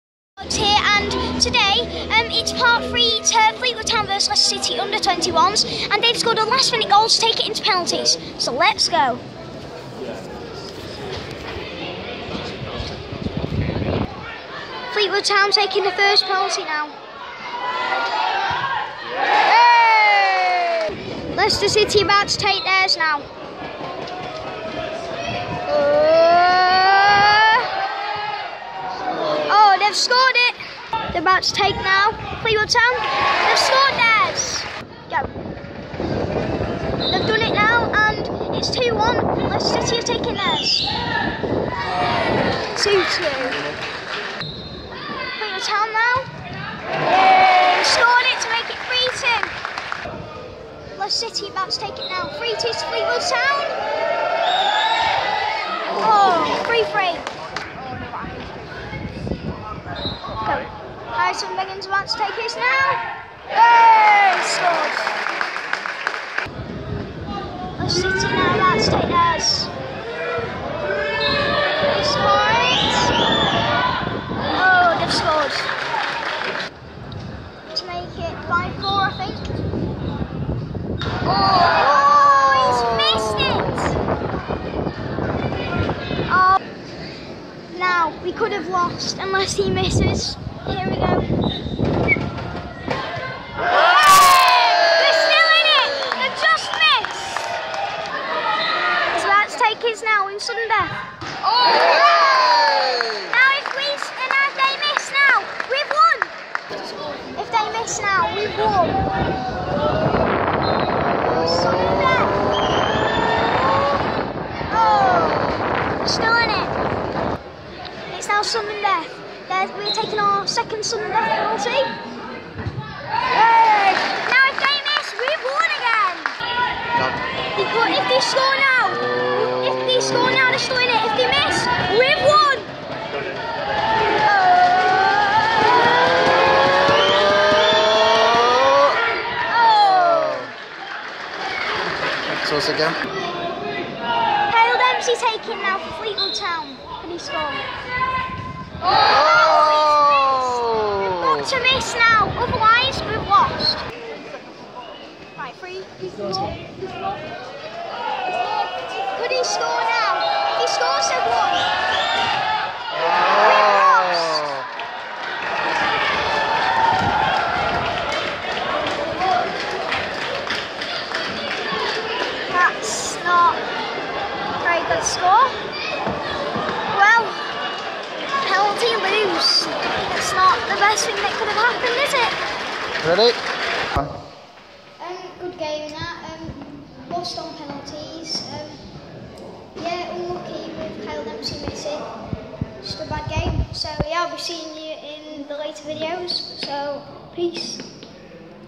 Fleetwood town vs leaster city U21 part 3 penalty shootout
FTFCVlogs 7-6 leaster city U21 played grate but a unlucky match for fleetwood